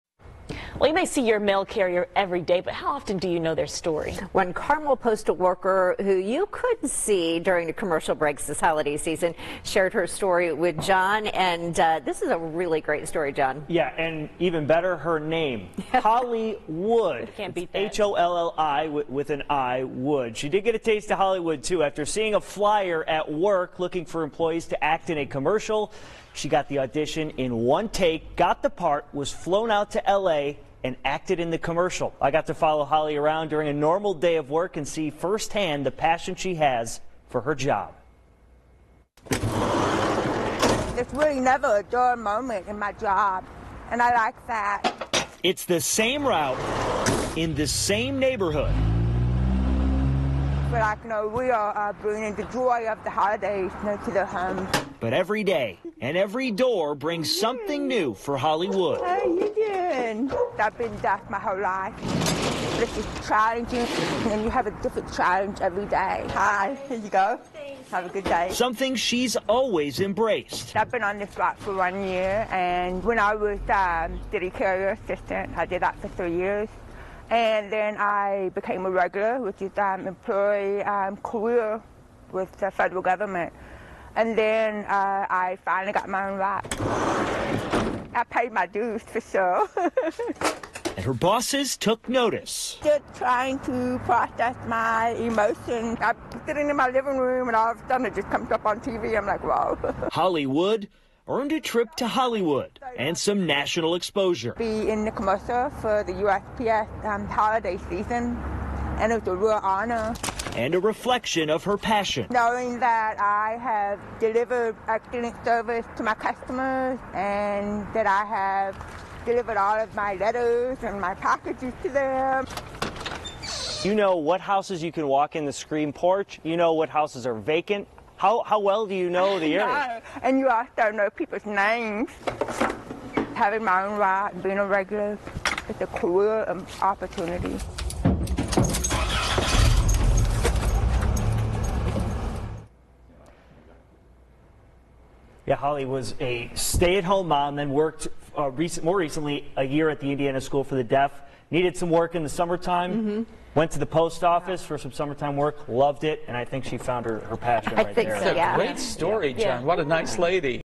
Carmel mail carrier Holli Wood goes to Hollywood
A deaf Carmel mail carrier earned a trip to Los Angeles to appear in a nationally televised US Postal Service commercial.